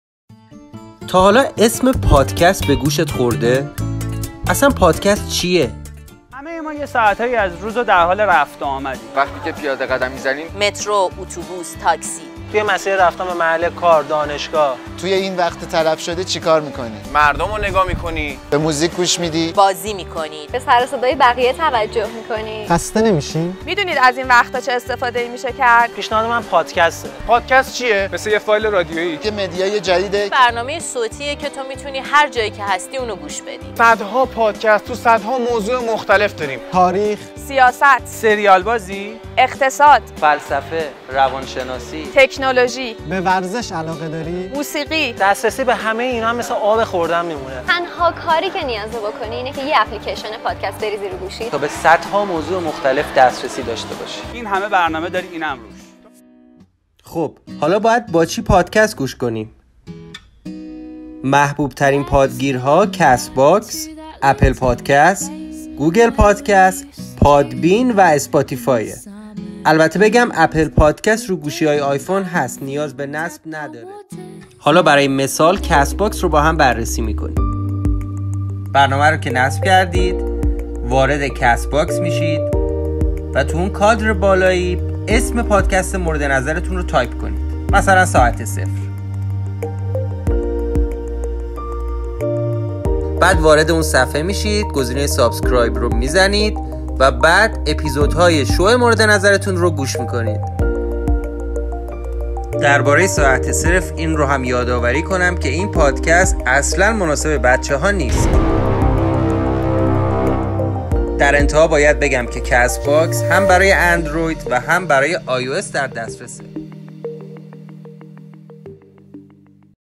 پادکست چیه؟
تا حالا اسمش به گوش تون خورده ؟
با پادکست های خوب فارسی آشنا هستید ؟
لطفا چند تا پادکست خوب رو معرفی کنید در کامنتها
محبوب ترین اپ های پادگیر:
Castbox
Apple Podcast
Google Podcast
Podbean